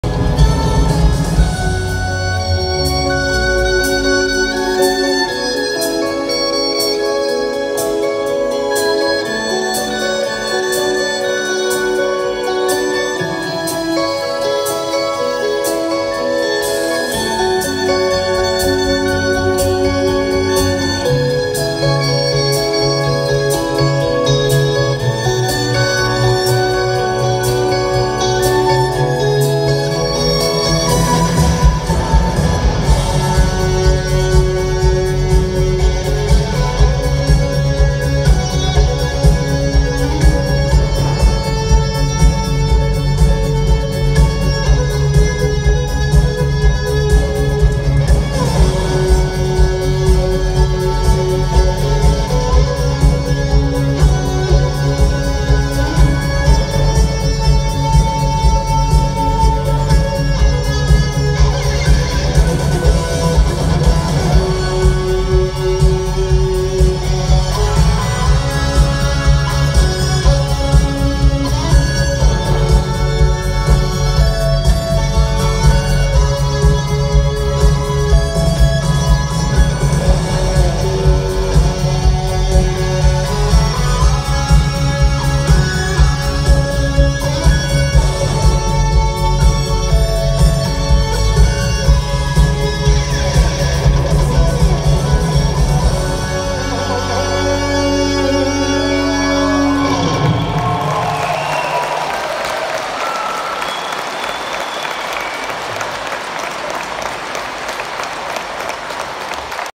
С днем рождения, Виктор Робертович! Спасибо за Все!
Симфоническое кино! Концерт в Ярославле, 2020 год!